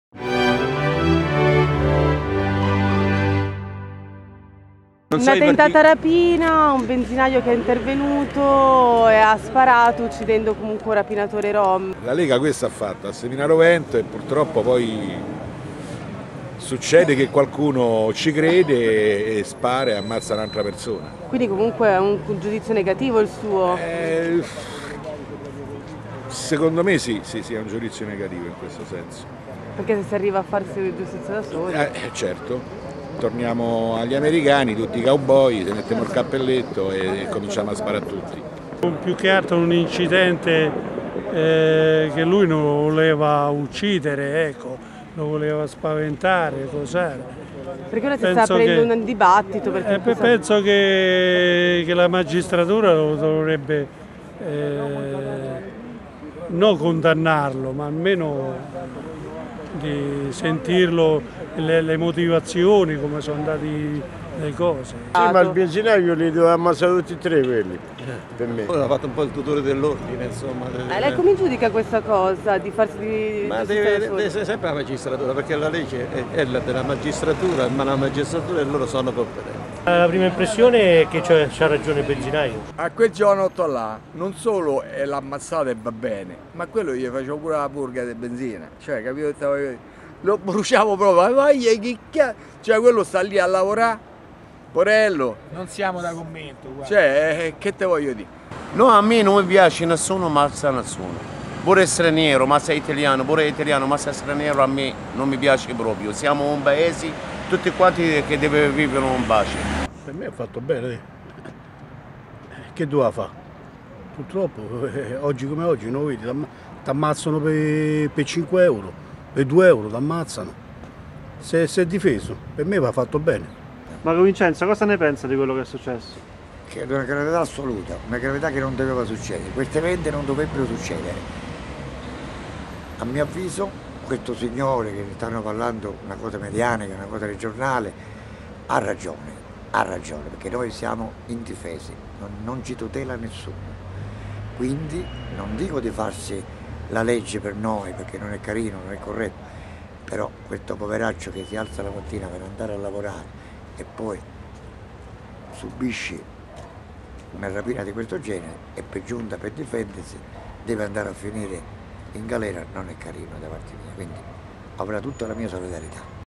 Tiburno.tv - Benzinaio spara ai rapinatori nel vicentino, cosa ne pensano a Tivoli e a Guidonia?
Indagato per eccesso di difesa Graziano Stacchio, il benzinaio veneto di Ponte di Nano, nel vicentino, che martedì sera 3 febbraio, ha esploso alcuni colpi di fucile contro dei banditi che tentavano di rapinare una gioielleria vicino al suo chiosco. Uno di loro, di origini Rom, muore. Il paese si sta mobilitando in suo favore, diventato un simbolo della "lotta contro l'escalation criminale" che sta sconvolgendo la zona.
Abbiamo chiesto un commento ai residenti di Tivoli e Guidonia su questo drammatico fatto di cronaca. Ecco le loro risposte.